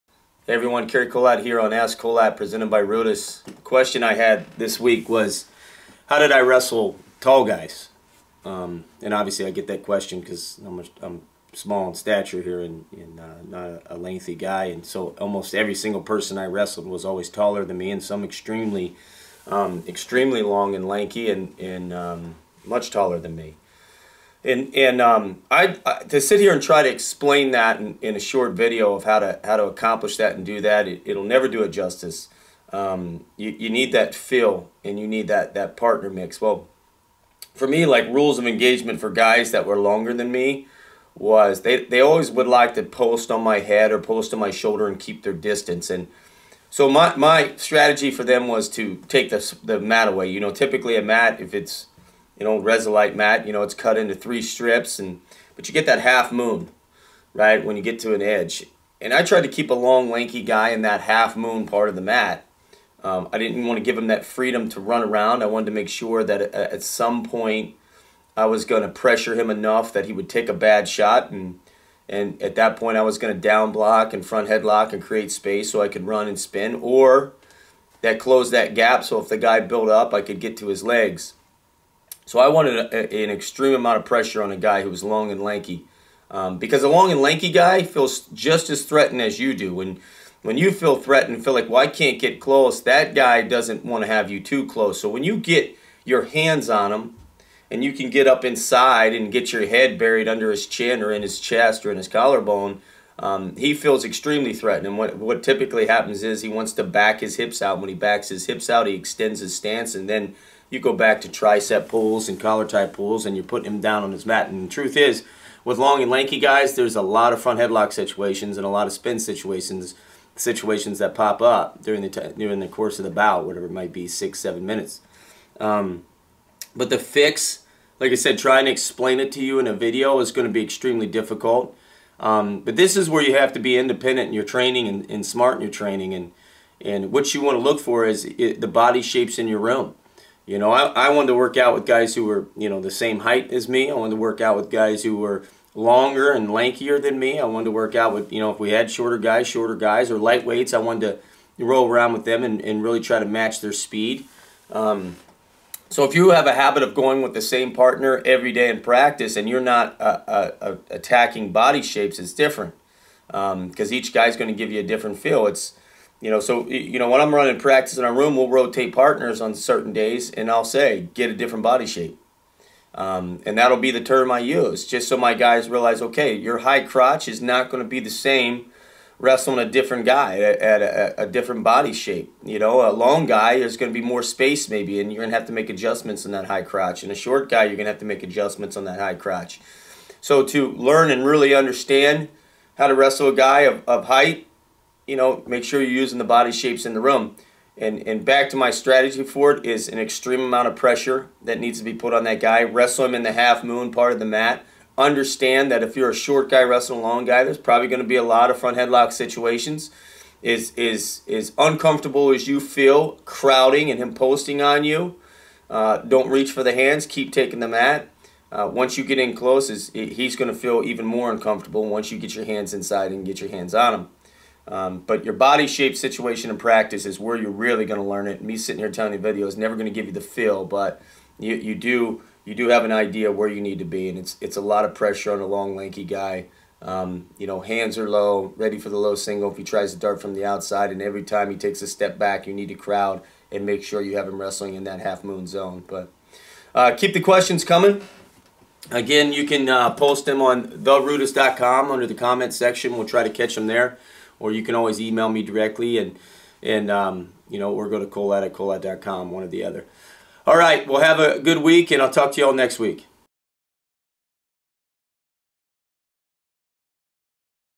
ASK KOLAT: How Do You Wrestle A Taller Opponent?
Passive Learning vs. Active Practicing
Cary Kolat addresses a question commonly asked of him throughout his career, "How do you wrestle an opponent with a height advantage?".  Almost every person Kolat wrestled against was taller with a longer reach than him. Merely explaining in a video how to wrestle a taller opponent is not enough to fully learn and implement these techniques. Seeking out a training partner to practice these approaches is the only way to truly build and execute these skills.

Wrestlers who were taller and longer than Kolat had a few commonalities and tendencies they tried to use against him. They would like to post on his head or shoulder and keep distance. Kolat's strategy was to take the mat away and apply extreme pressure. A typical and classic mat is cut into three strips. His strategy was to pressure them into staying within half the circle and not allow them to run around. Every time they take a step back he would step forward and crowd them. This would force his opponent to take a bad shot. He could then down block and front headlock or close the gap so he could get to his legs. A taller opponent feels just as threatened as you do. With tall opponents, there are a lot of opportunities for font headlock and spin situations. As uncomfortable as you feel with your opponent posting on you, don't reach for the hands keep taking the mat.

Choosing Wrestling Partners
In order to fully learn these techniques, you have to seek out a longer, taller wrestling partner. During Kolat's career, he continuously chose to wrestle and drill with all types of wrestlers. He would practice with taller guys or against lighter guys who were faster and try to match their speed. This is so important Kolat uses it during college practices. He'll tell his wrestlers to switch partners and specifically to find a different body shape. It forces you to realize the adjustments you're going to need to make against different body shapes. Overall, learning what strategies to use is great but it means nothing if you're not practicing them before you're against a taller opponent in competition.

Check out all RUDIS media including:
Mental Mondays
RUDIS Wrestling Podcast
ASK KOLAT
The Way: A RUDIS Wrestling Podcast